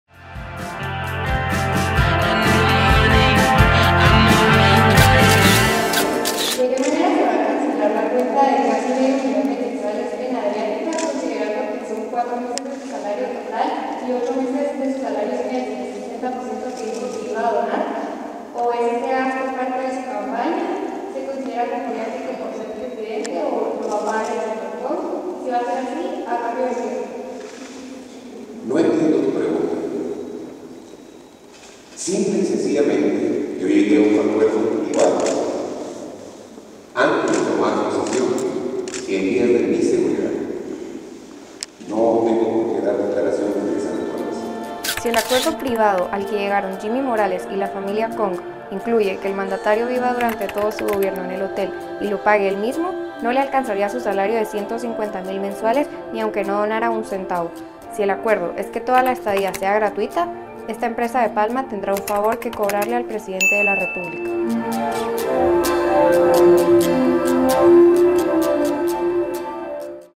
Jimmy oculta quién pagará la cuenta de 100 días en el hotel de un palmero
El presidente ‘contra la corrupción’ no quiere transparentar sus deudas. Jimmy Morales se niega a hacer público quién pagará su cuenta, que va por medio millón de quetzales, por vivir casi 100 días en un lujoso hotel en la zona más exclusiva de la Ciudad de Guatemala.